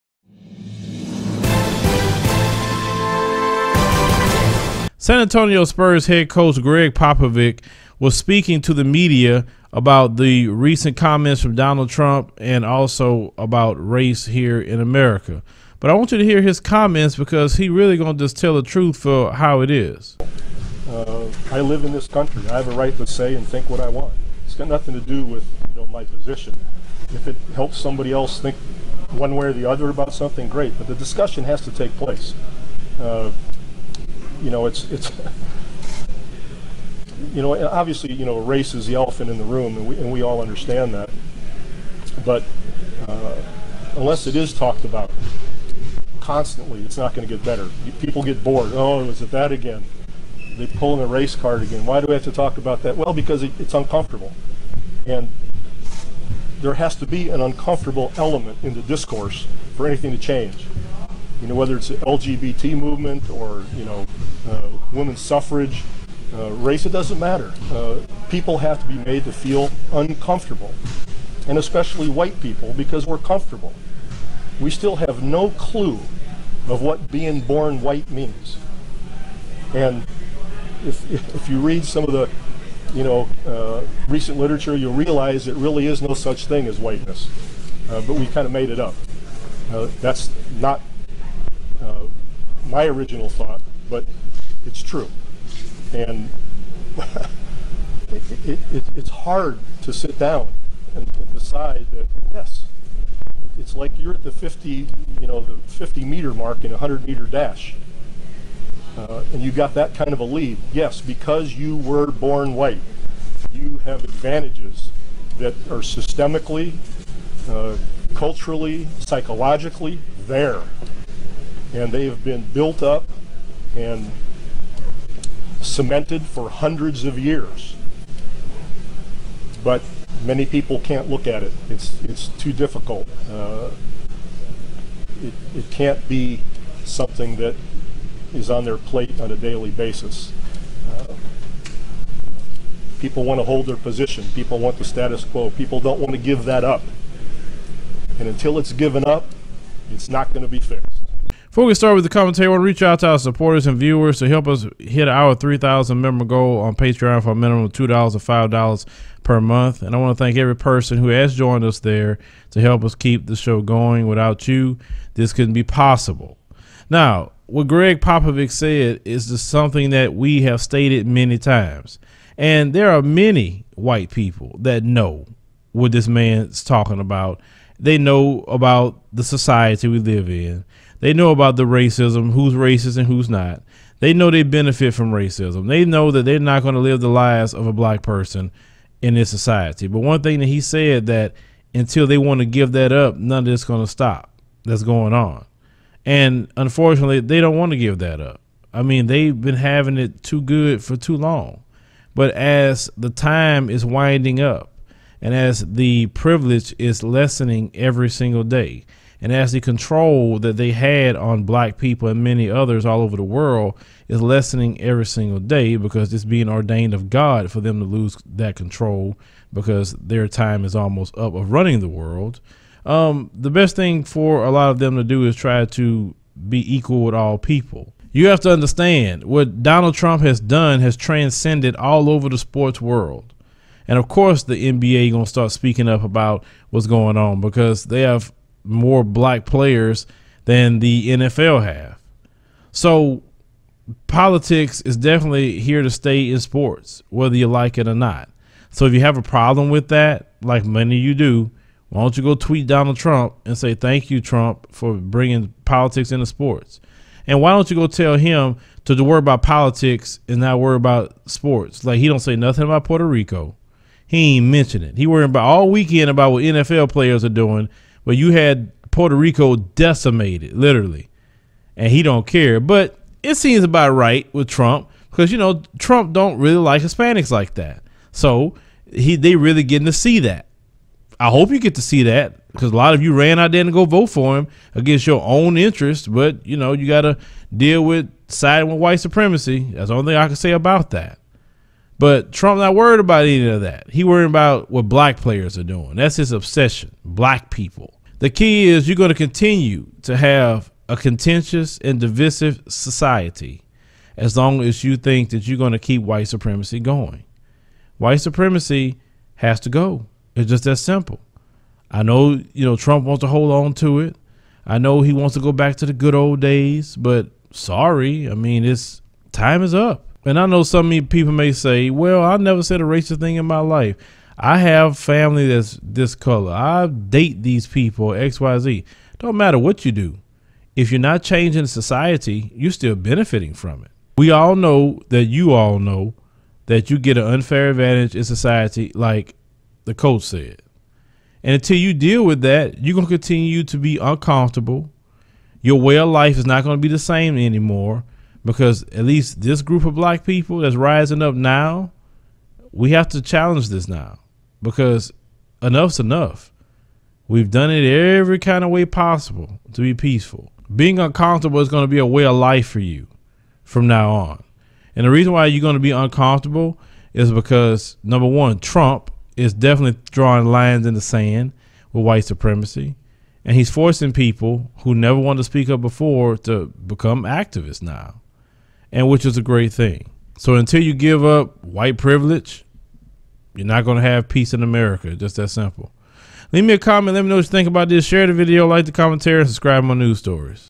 San Antonio Spurs Head Coach Greg Popovich Says White People Must Become Uncomfortable In America
Advise Media Network
2323 Clear Lake City Blvd. Suite 180 Box 133